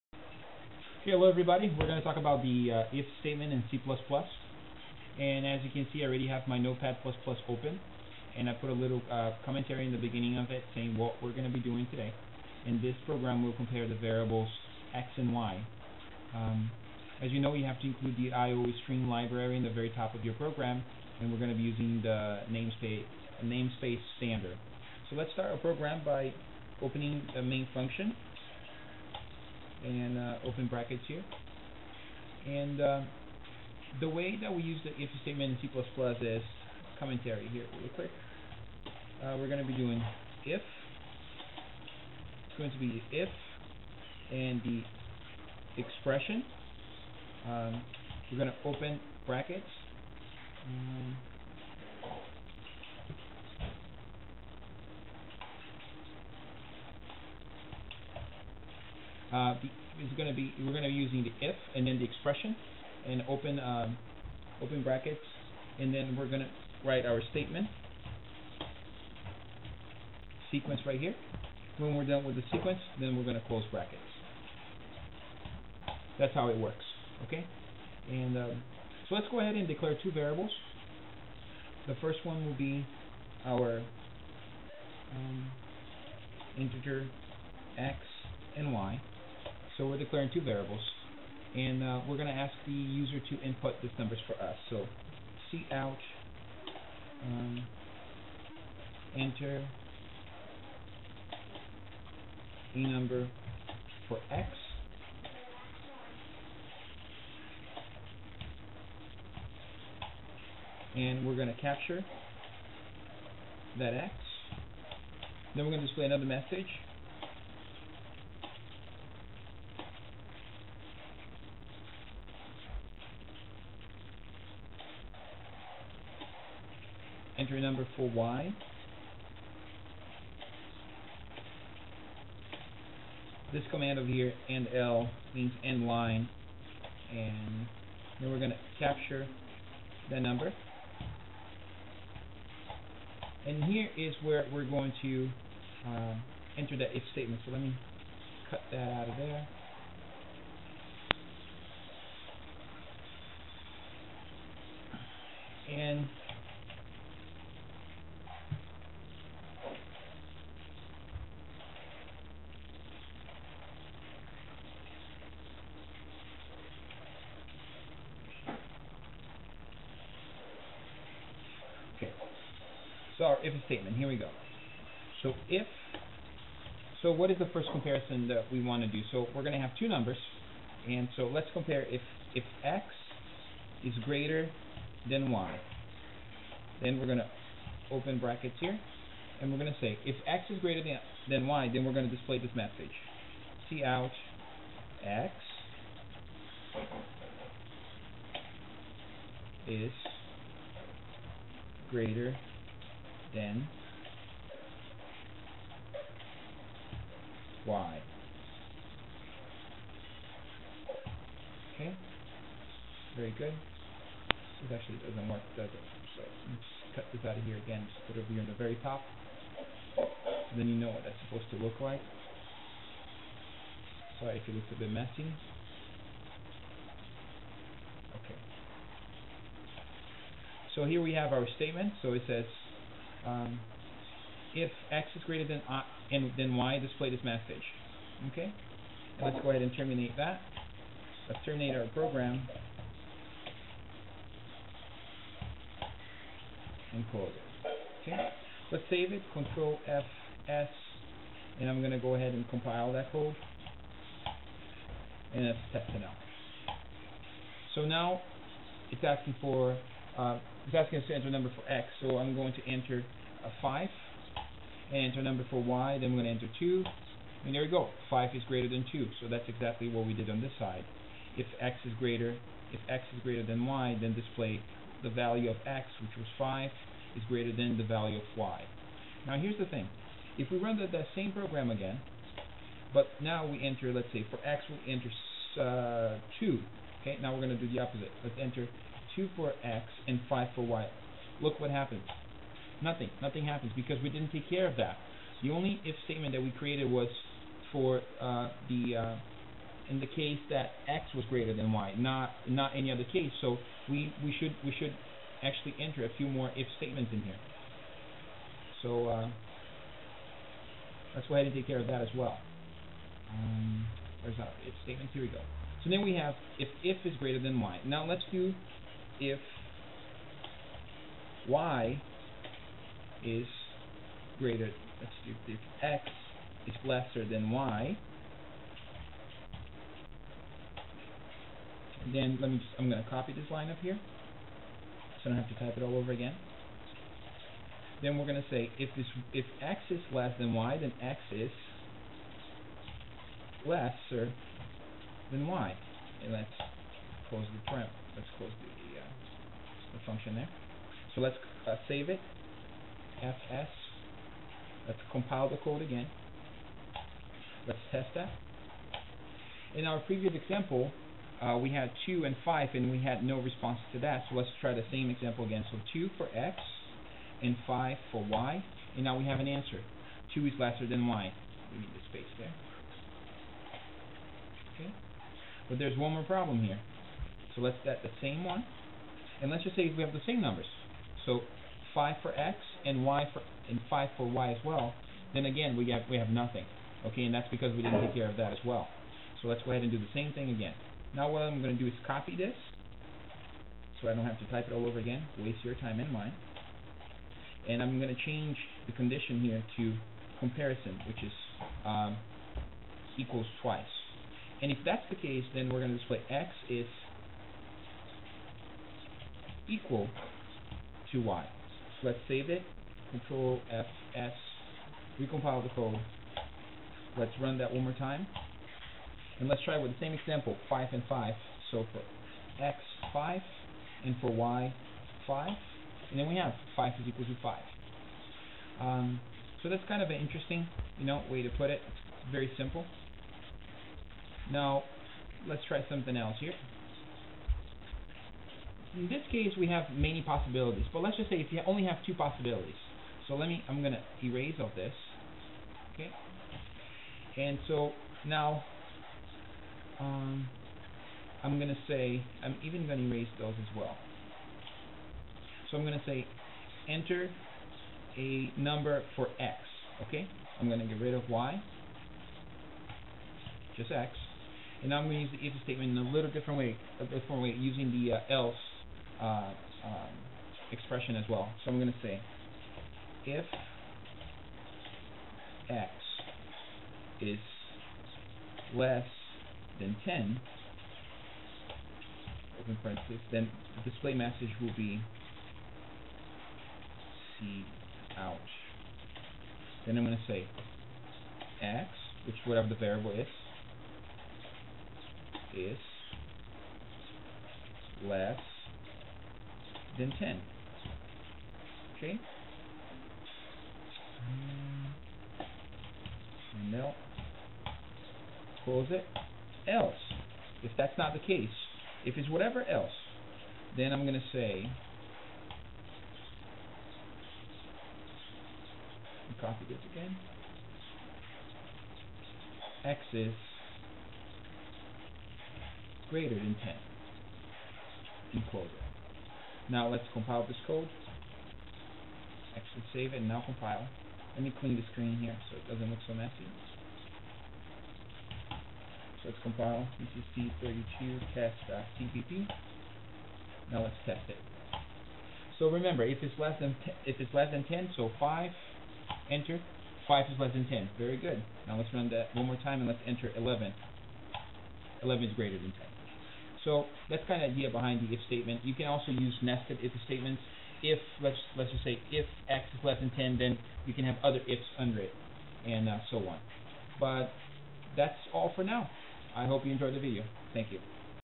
C++ Tutorial - The IF statement
A breif description on how to structure and use the IF statement in C++ by Fabio Scopel